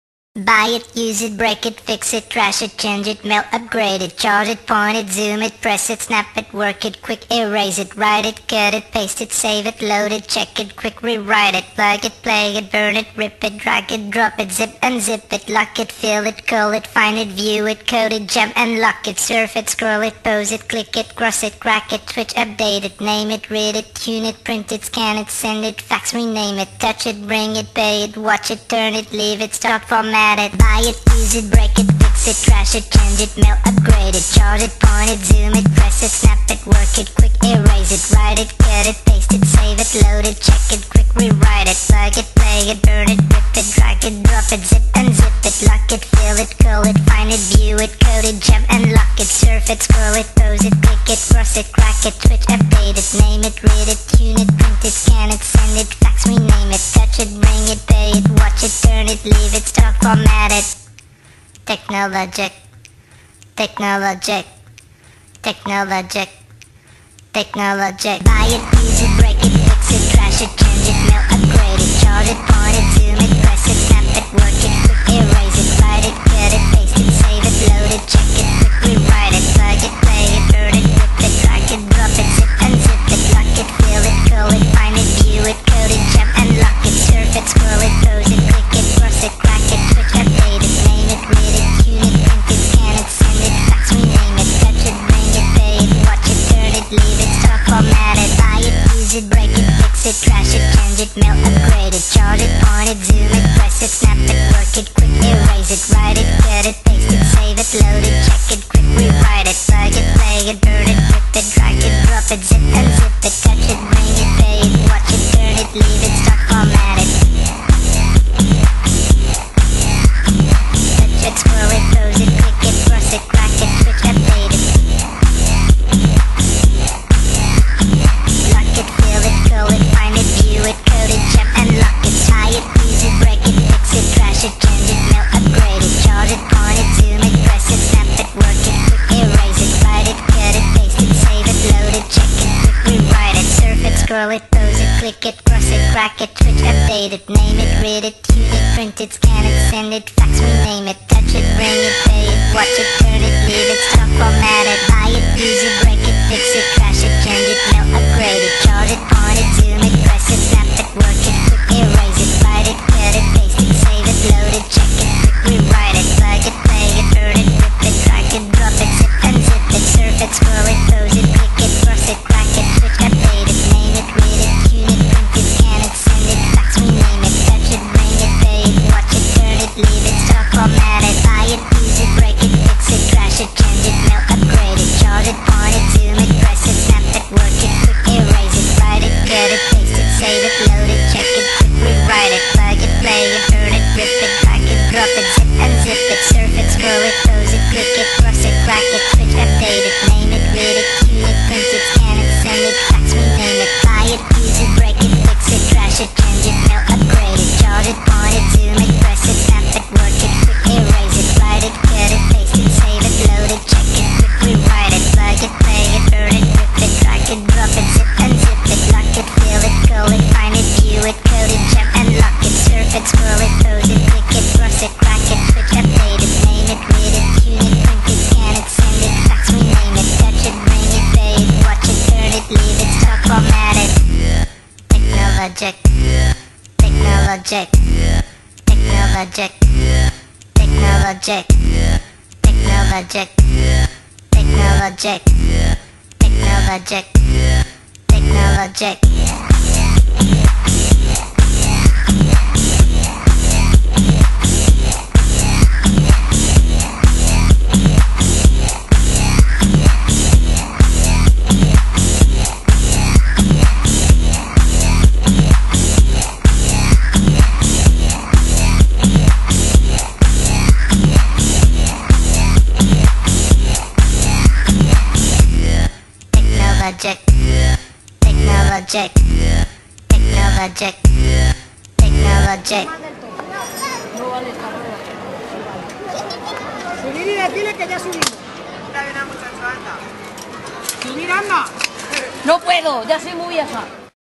Carrera de Camas - La Villa de D.Fadrique (Toledo) Sept.08
Carrera de camas en la villa de don fadrique, organizada por oldville pub. sept. 08. Grabado y montado por Polonio Muñoz.